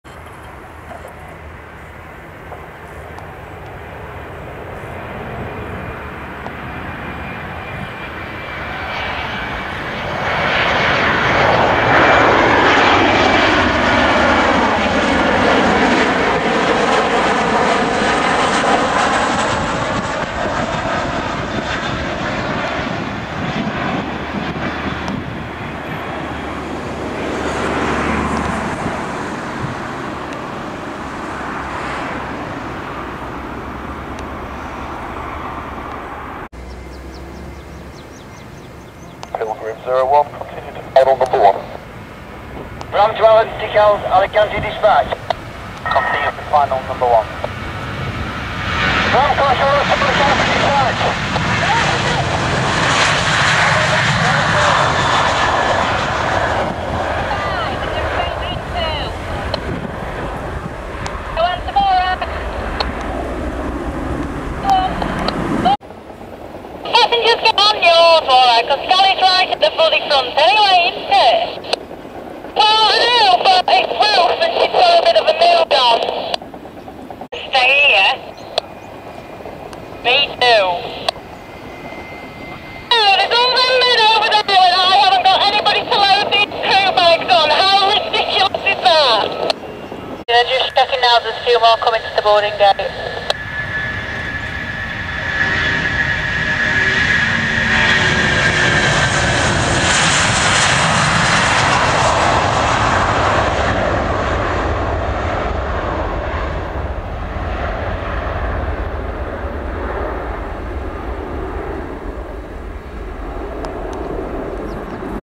RYANAIR & THOMSON TAKE OFF & LANDINGS @ DONCASTER AIRPORT (UK)
No67 VIDEO OF THOMSON B737 LANDING FROM FARO, RYANAIR B737 DEPARTING TO GIRONA & THOMSON B757 DEPARTING TO MALLORCA MON 16th JULY BY M CONNOLLY.